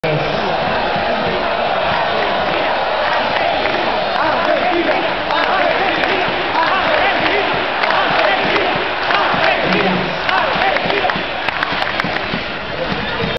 Copa Davis 2006 Argentina vs Australia 3
Partido de semifinal de copa davis entre Argentina y Australia. Sector VIP. tucu4u